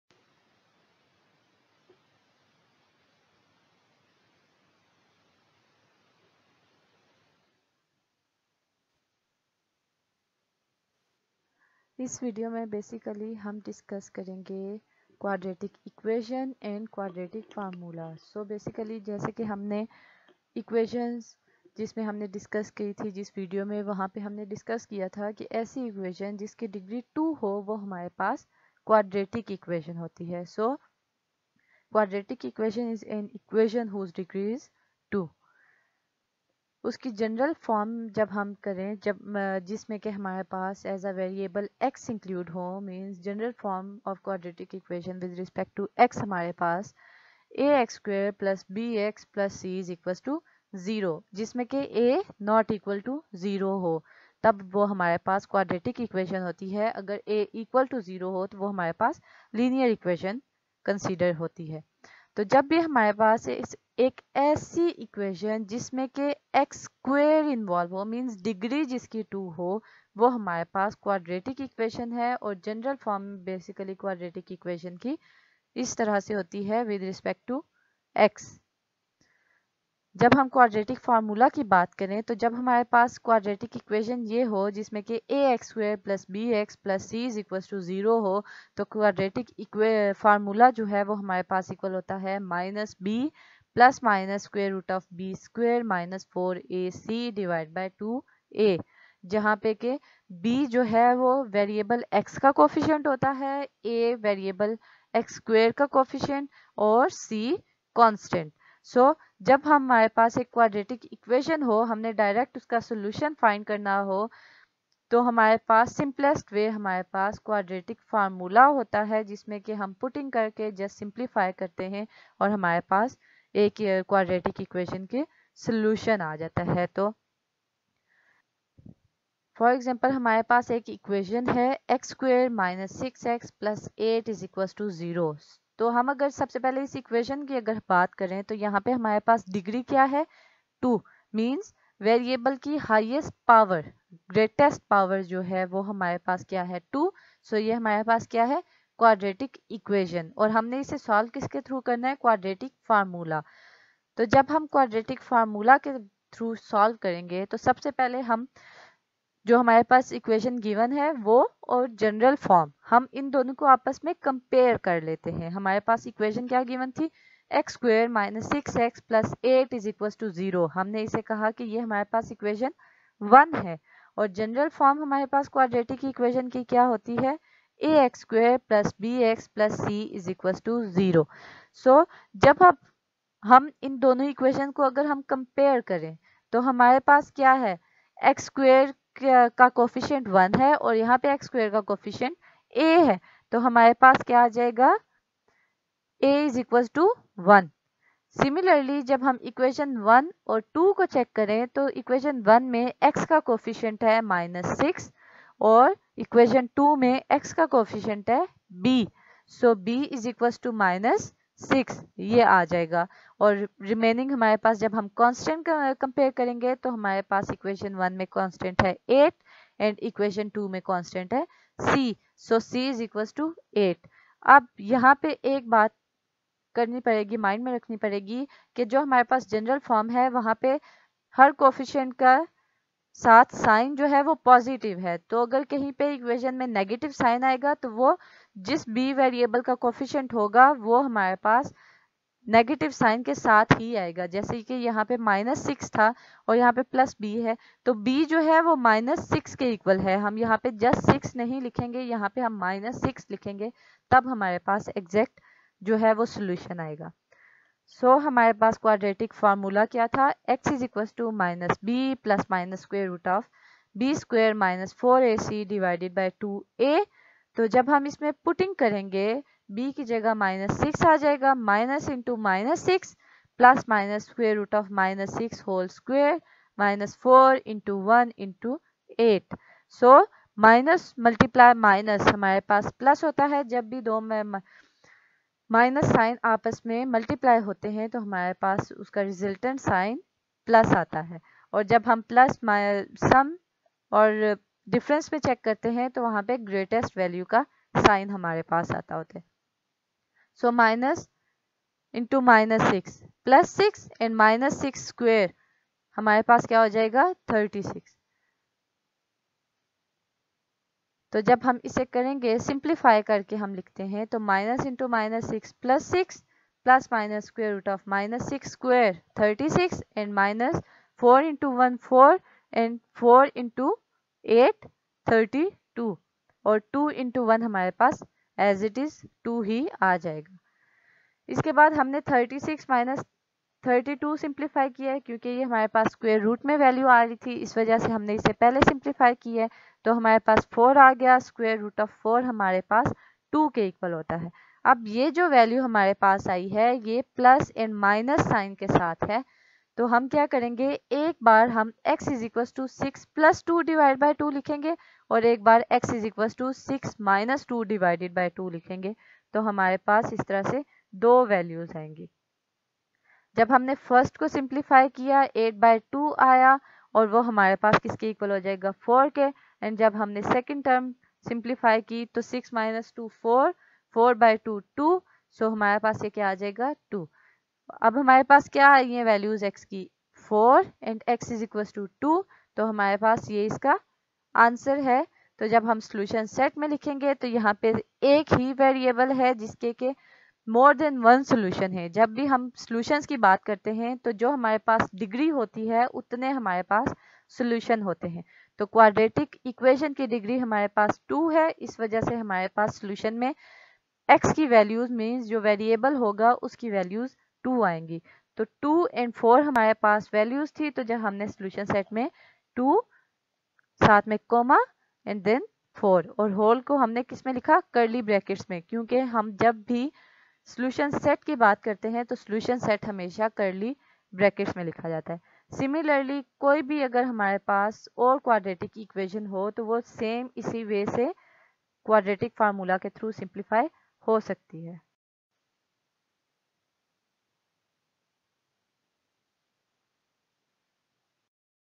Quadratic formula
Quadratic Formula is short method to find solution of quadratic equation.